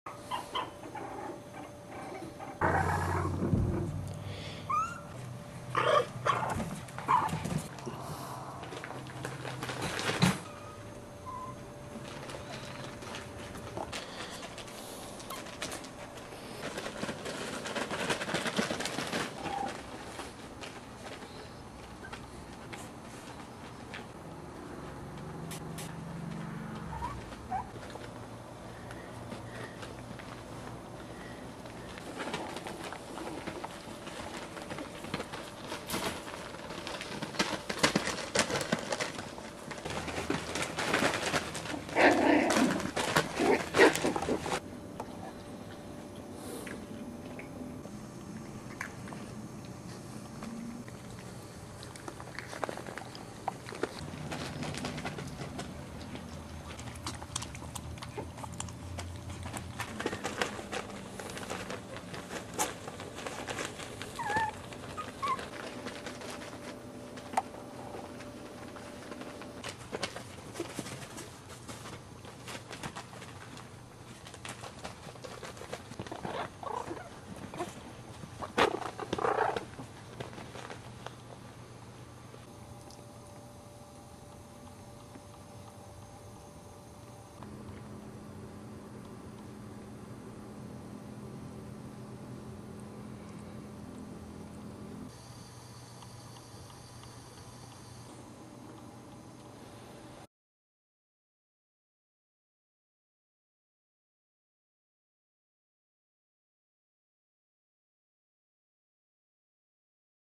Chihuahua Puppies First Day Home (in HD)
Chihuahua puppies first day home from the animal shelter and they meet our Maltese dogs.